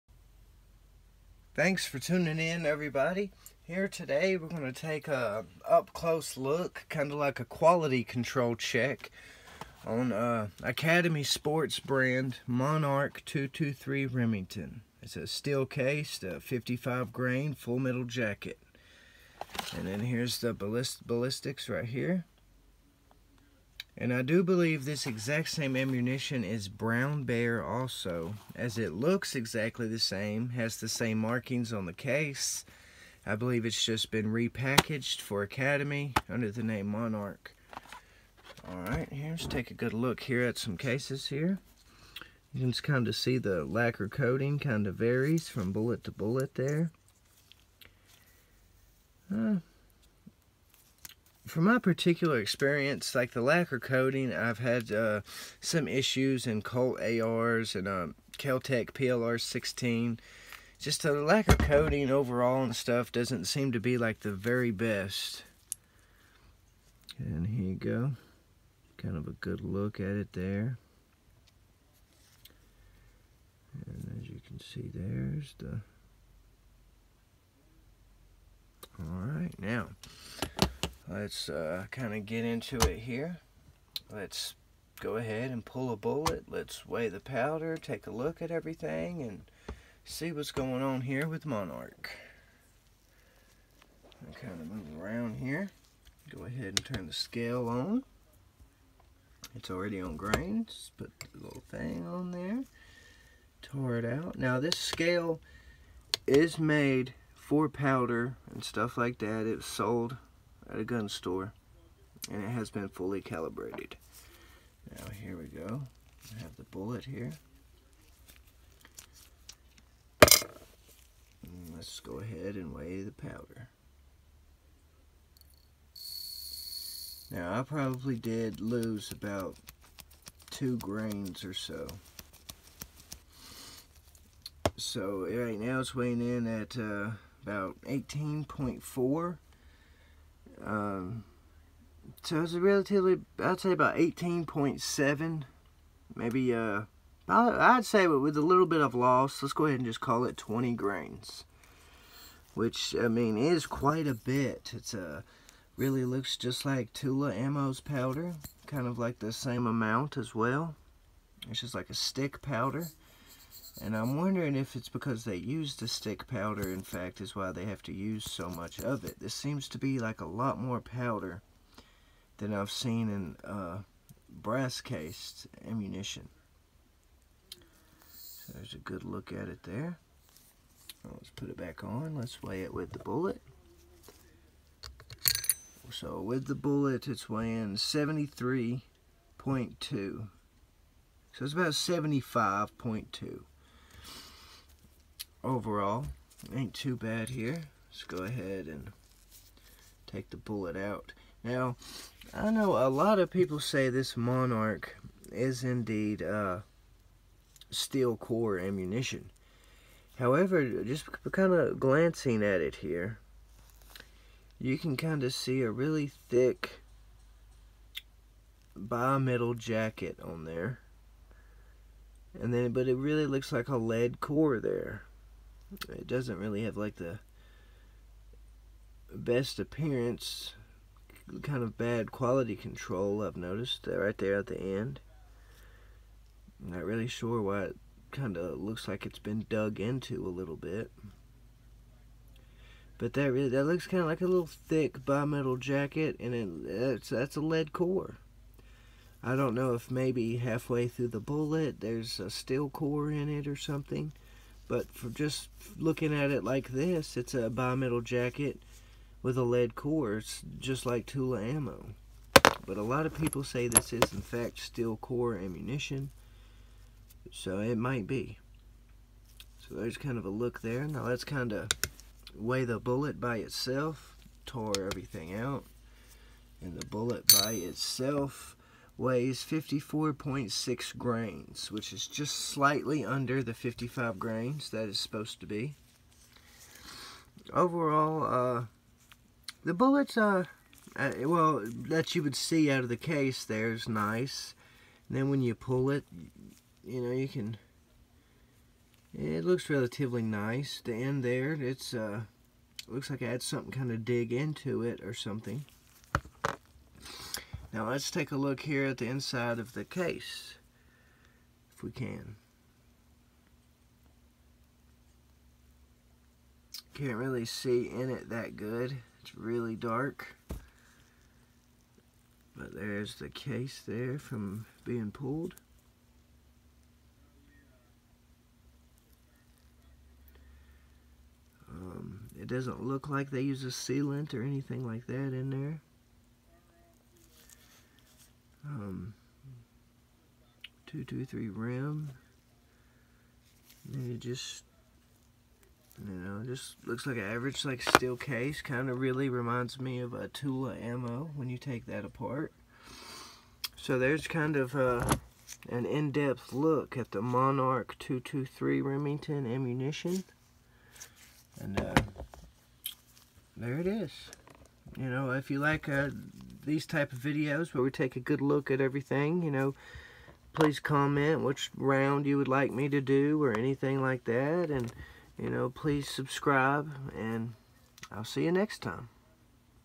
Monarch 223 55gr fmj an in-depth look
A in-depth look at monarch 223 55gr fmj I pull the bullet and basically do a quality check